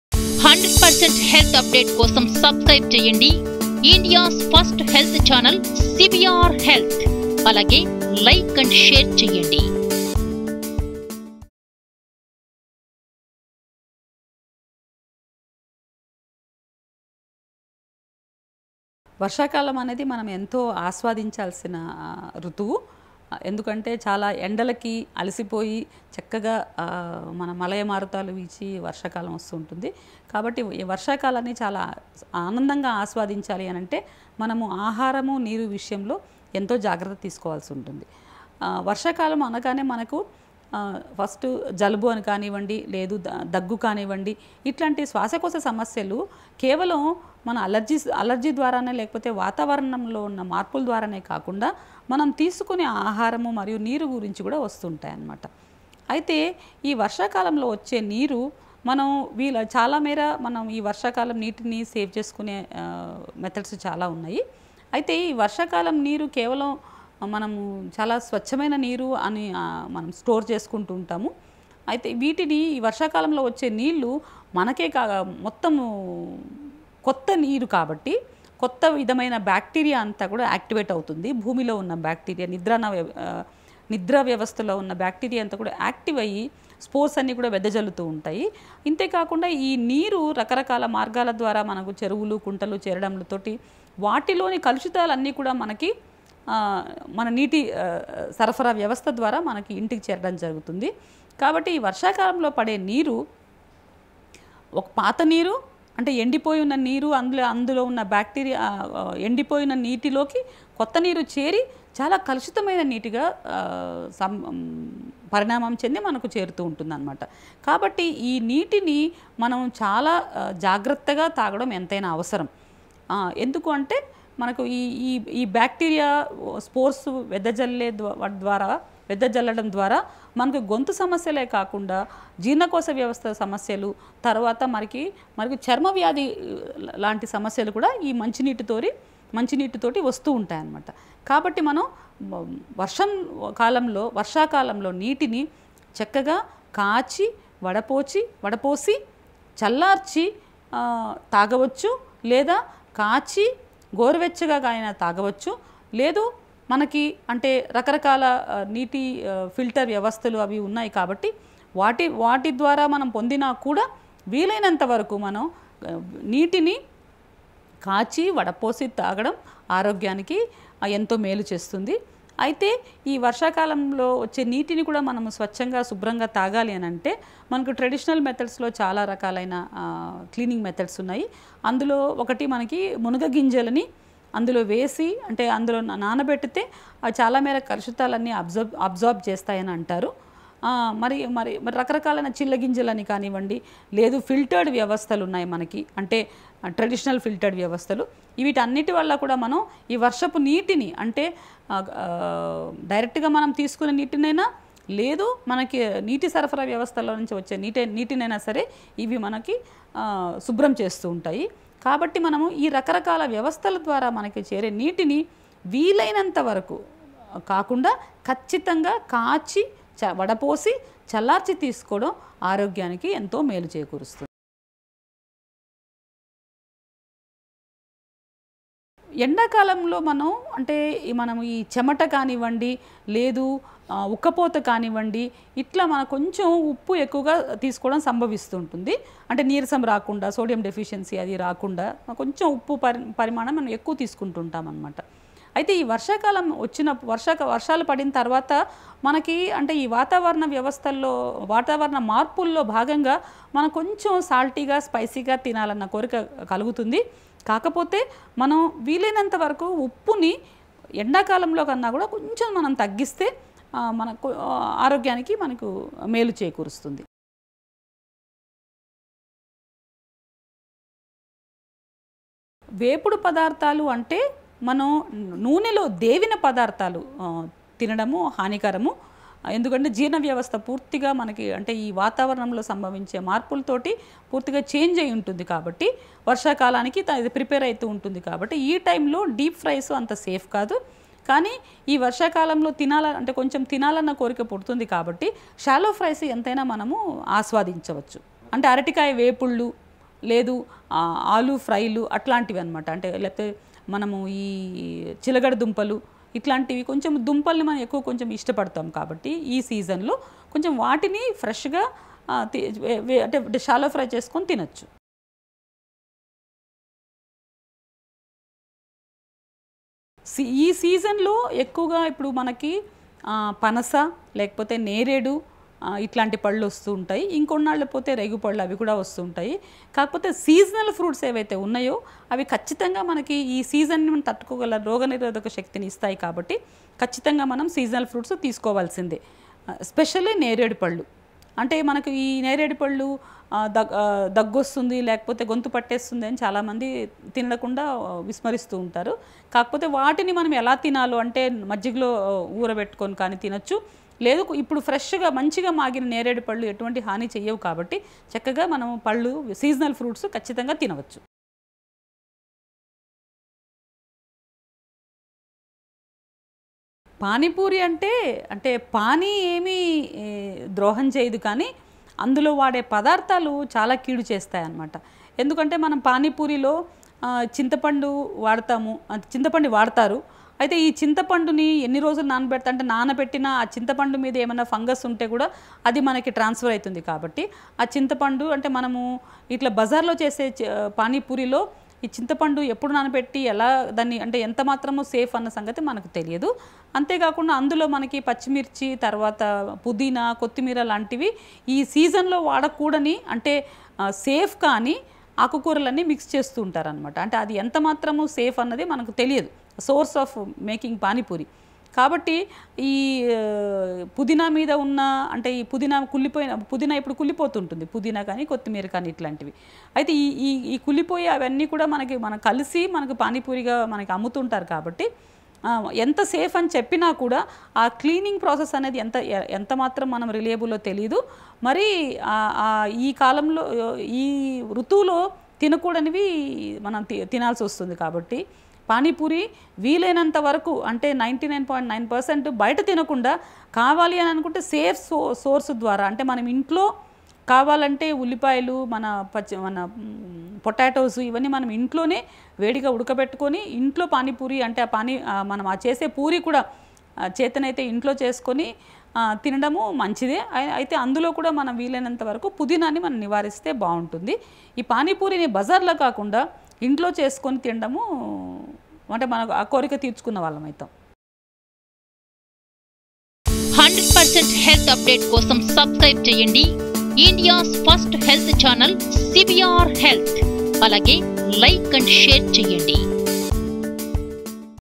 Precautions to be Taken During Rainy Season | Food & Water | Health Precautions |CVR
Precautions to be Taken During Rainy Season | Food & Water | Health Precautions| Rainy season | CVR Health

Dr. Nagalakshmi Speaks About Diet in Rainy Season.